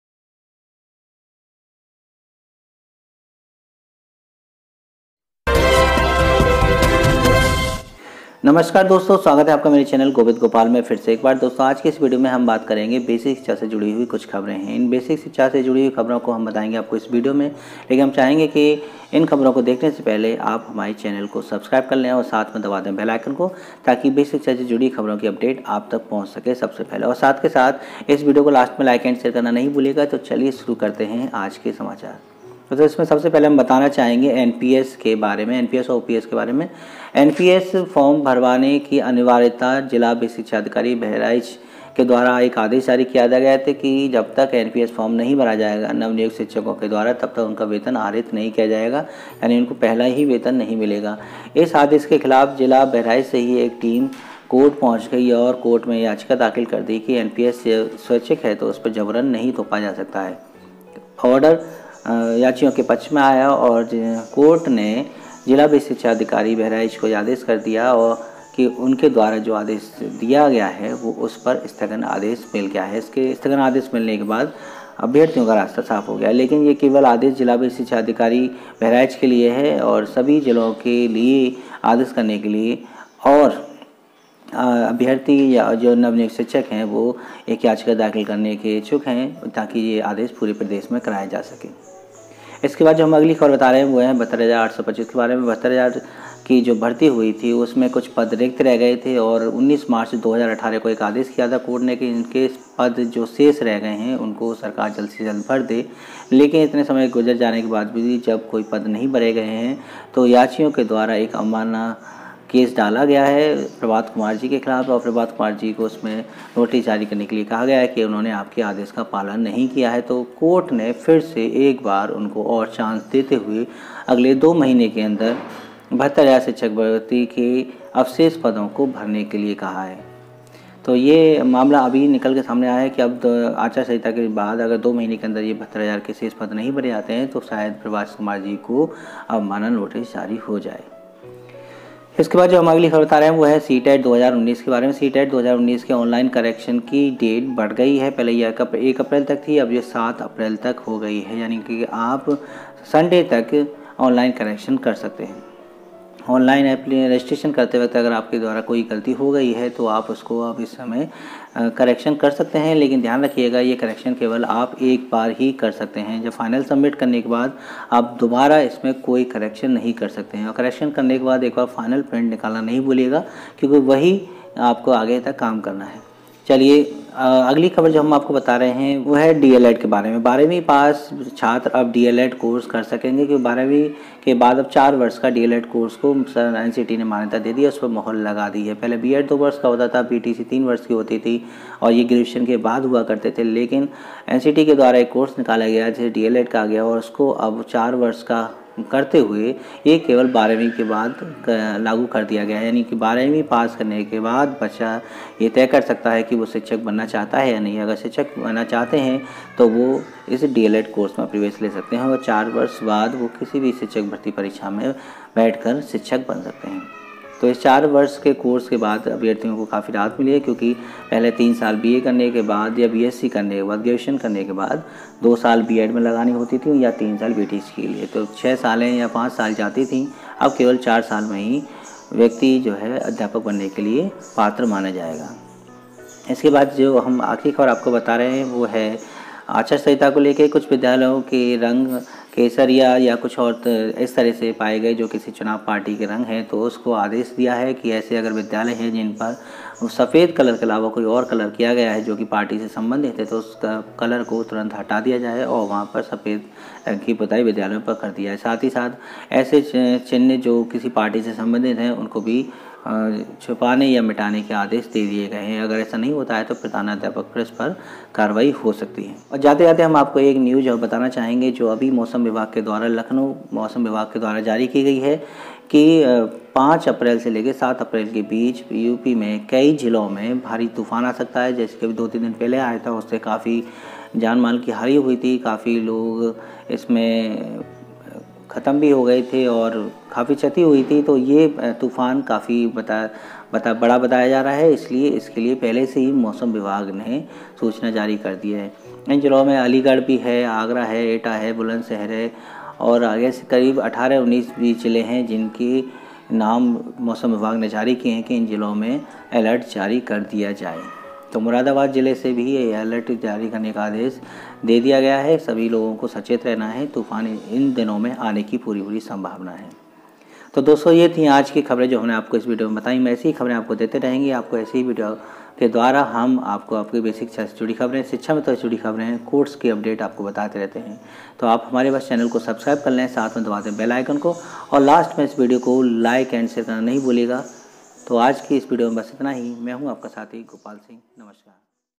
72825 शिक्षक भर्ती, NPS पर कोर्ट का स्टे और CTET की अपडेट संग 5 खबरे।
शिक्षामित्र से जुड़ी खबरों को देखने के लिए नीचे क्लिक कीजिए

Hi friends..
Is video me 72825shikshakbharti, NPS OR CTET KI news update hai..